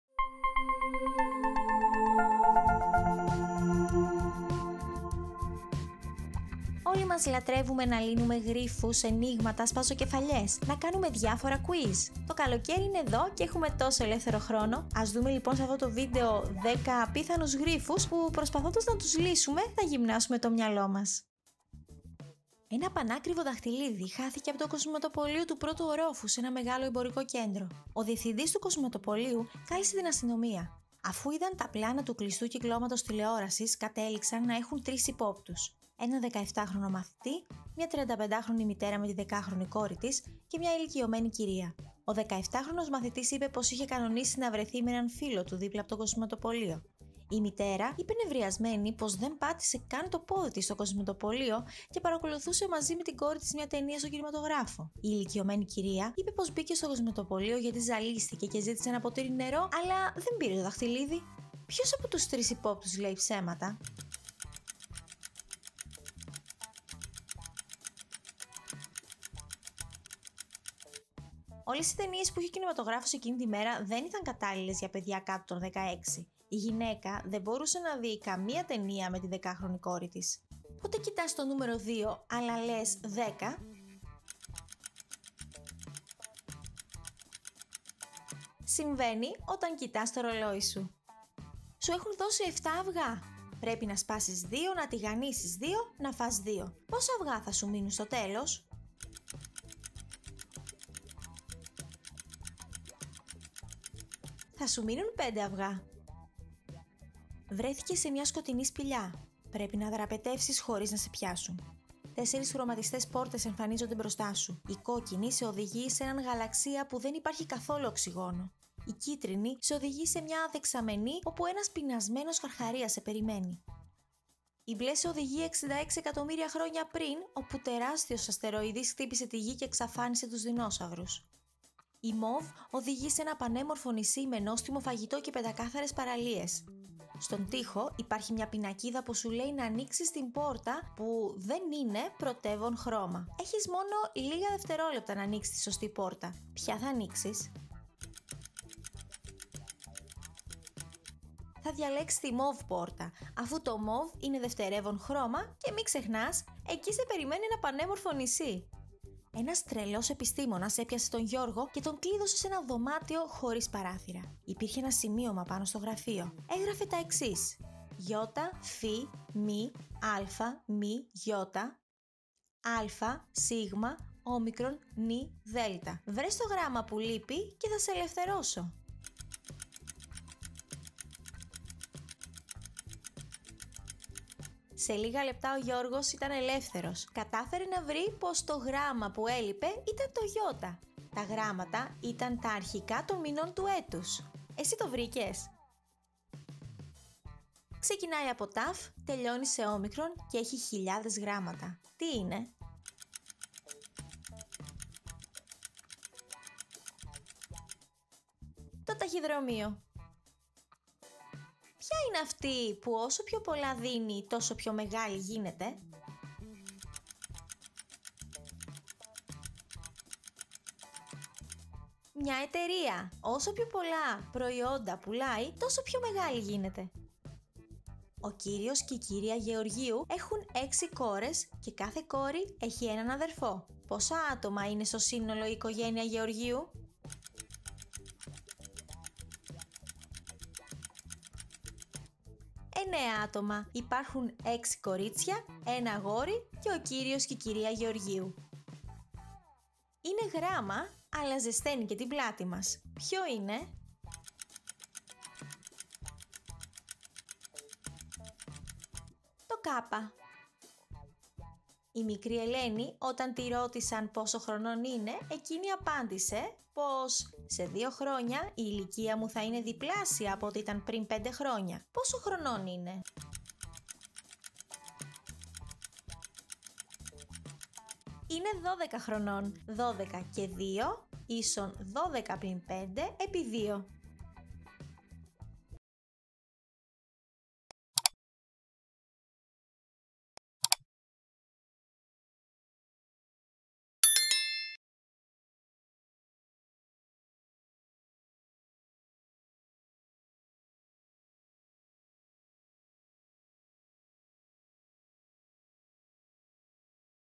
10 ΑΠΙΘΑΝΟΙ ΓΡΙΦΟΙ για να γυμνάσεις το μυαλό #3 - Τα Καλύτερα Top10
Όλοι μας λατρεύουμε να λύνουμε γρίφους, αινίγματα, σπαζοκεφαλιές! Να κάνουμε διάφορα κουίζ! Το καλοκαίρι είναι εδώ, έχουμε τόσο πολύ ελεύθερο χρόνο, γιατί λοιπόν να μη λύσουμε μερικούς γρίφους;